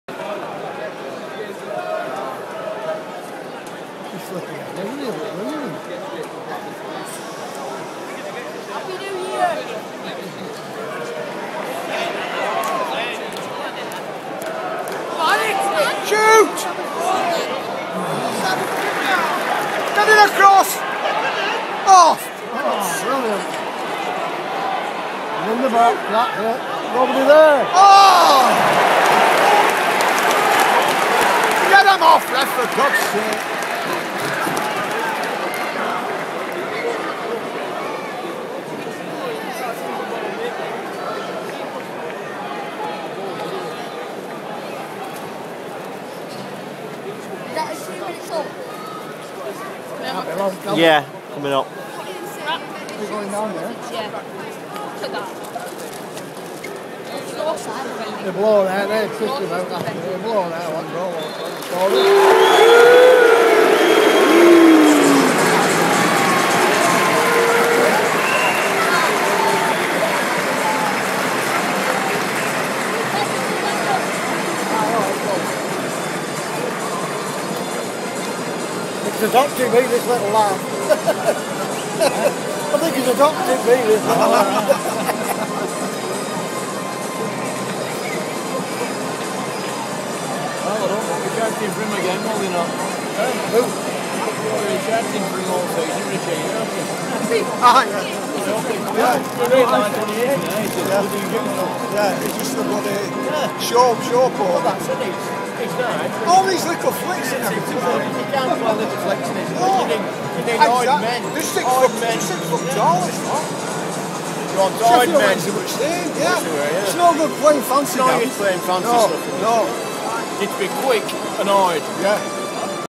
Blackburn Rovers vs Birmingham City - Blackburn Booed Off at HT - 29/12/2013
This was Blackburn Rovers being booed off at Half Time of the game between Blackburn Rovers vs Birmingham City. Blackburn lost 3-2.